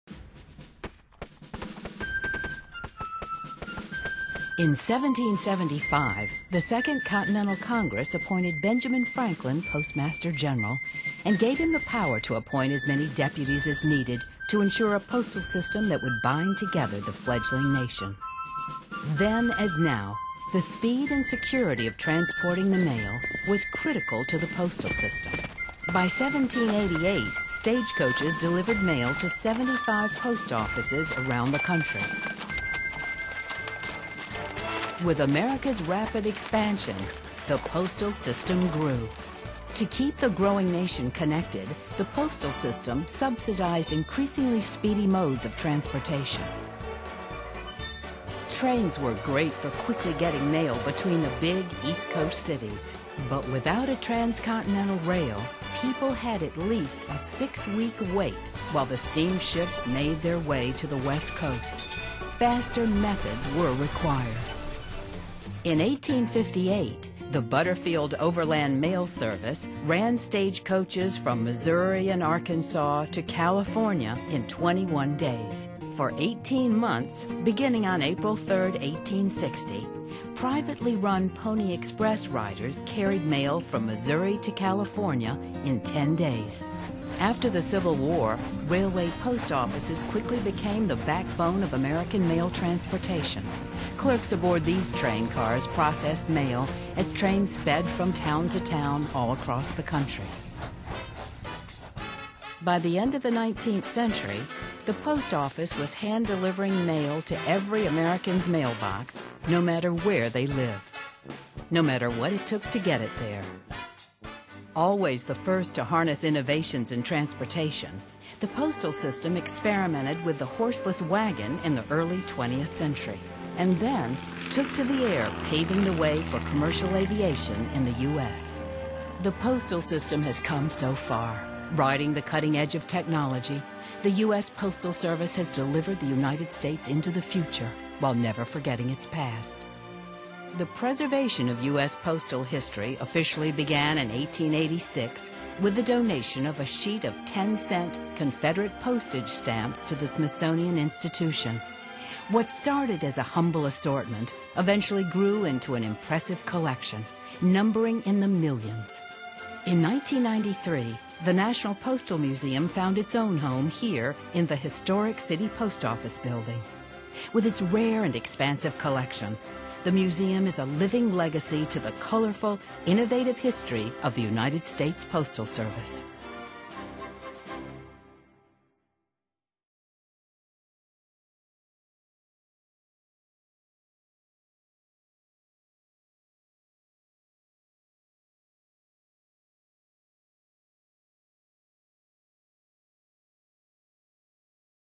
National Postal Museum Finds Its Home
A short video about the background to the Smithsonian Institution's preservation of postal history and the creation of the Smithsonian National Postal Museum.